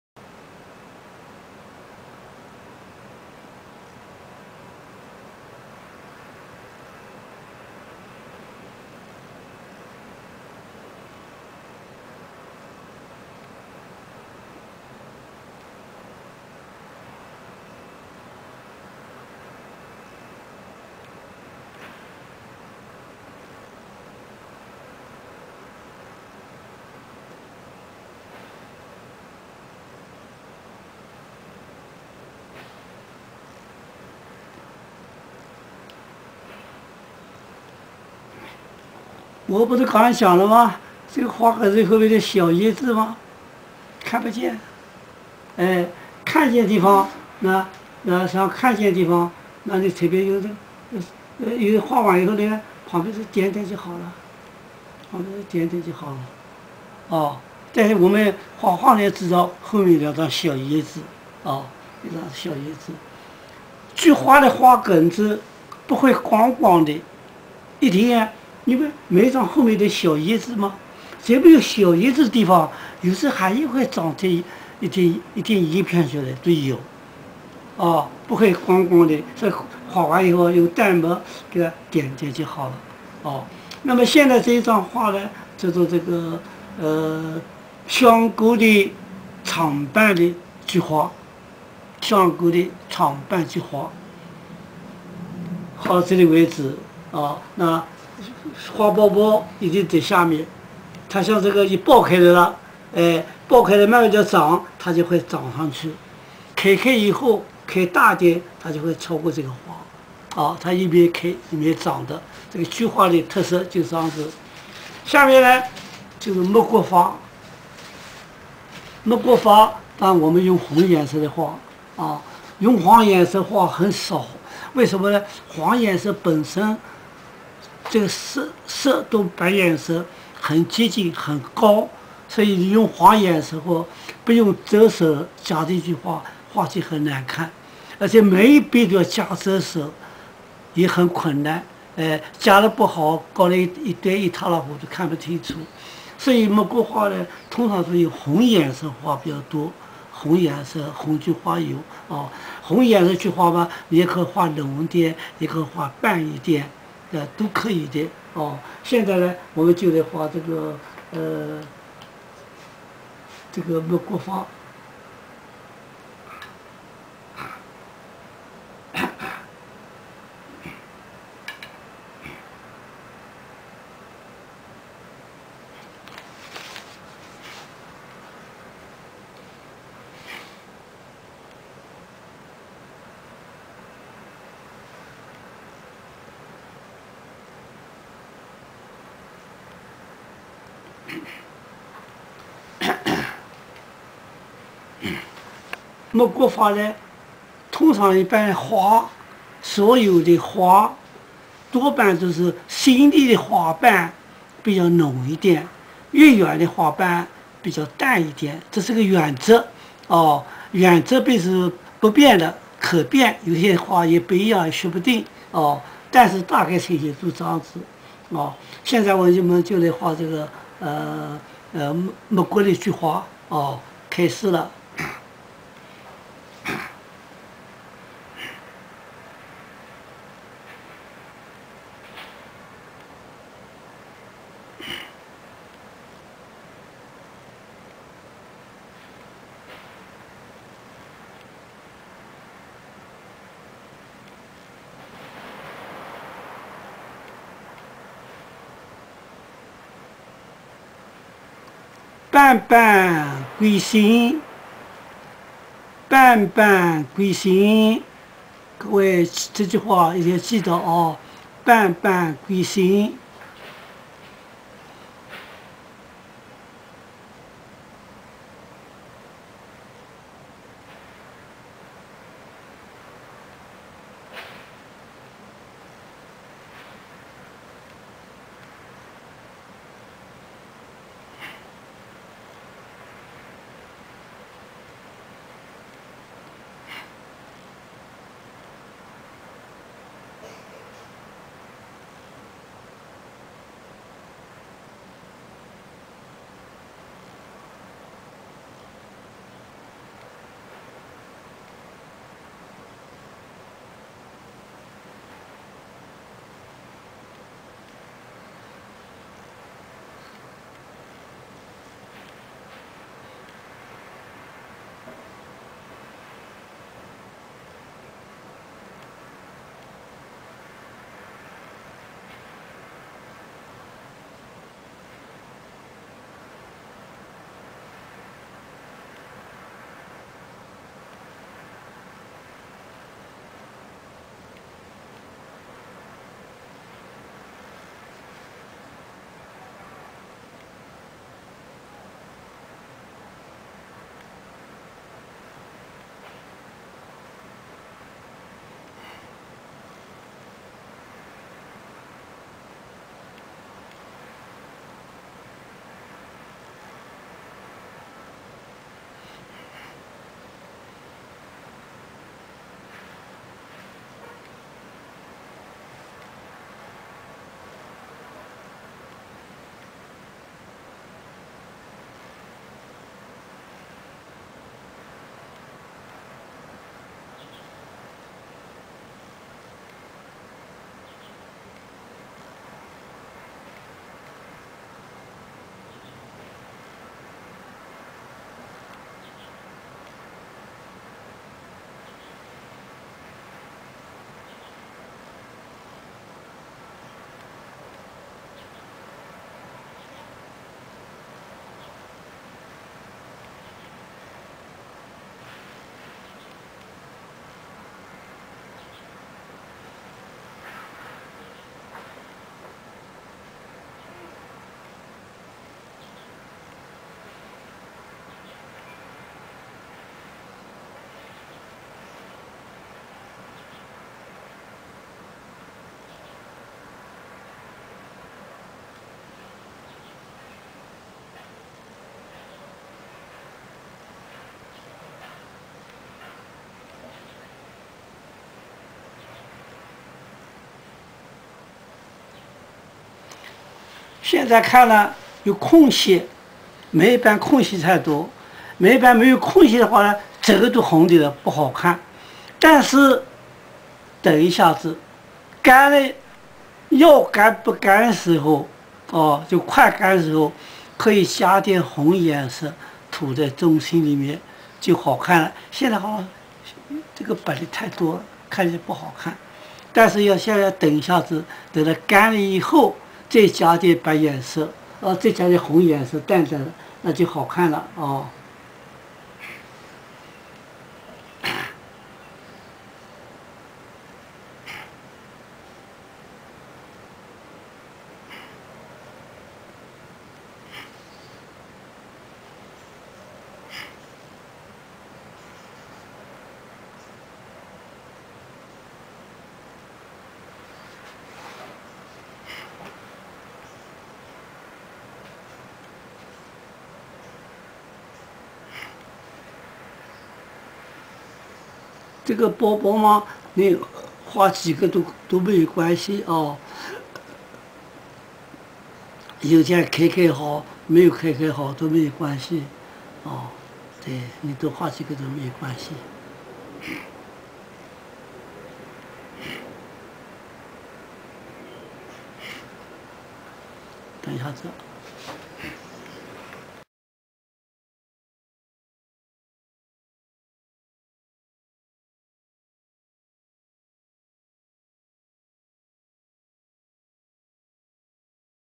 33.國畫自學-花草-菊花-8/10
33.國畫大師倪汝霖談國畫自學-花草-菊-8
On chrysanthemum's stalk, the light will be uneven, certainly will have the small leaf. The small leaf on the leaf comparison blank's stalk, with stipples the expression. Writes hollow-center strokes the drawing technique the long petal to complete chrysanthemum to this.
 The below lecture of long petal colored ink cancels the law, with red draws. Draws and the white color with the yellow is very close, must increase brown draws, otherwise draws is very ugly. But each uses the increase method, is not easy, if has moved, is also not good.
 Therefore the ink outlines the law, usually draws with the red color, may use the thick spot, or the pale spot draws, may.  Now uses the ink to outline the law, the ink outlines Fathom often all flowers, the new flower petal, the color with is quite thick, is farther, the pigment uses to be quite pale, this is a principle. Now the picture red flower petal, each flower petal to the colored heart, this point must pay attention. Now looked that the flower petal has the crevice. If the flower petal does not have the crevice, entire is red, is ugly. But, when soon dry time, may add a points red color, spreads inside the center, can be attractive. Using this time, draws the quite small flower and the calyx first under.
Shutechen 20100711